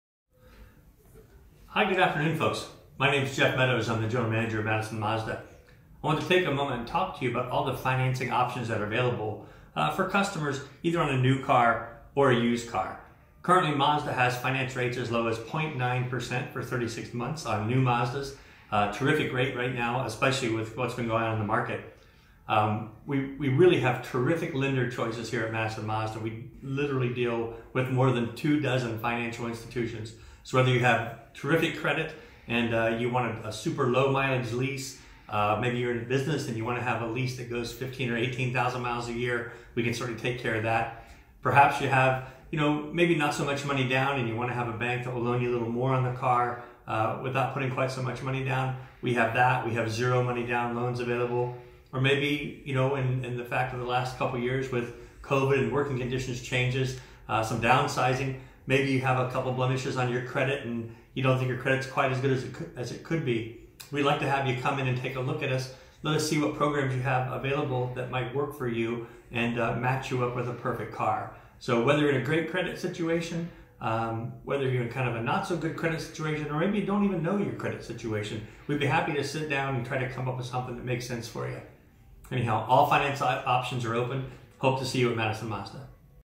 Financing Options with Russ Darrow Mazda Madison
Jeff explains the financing options available for all new customers!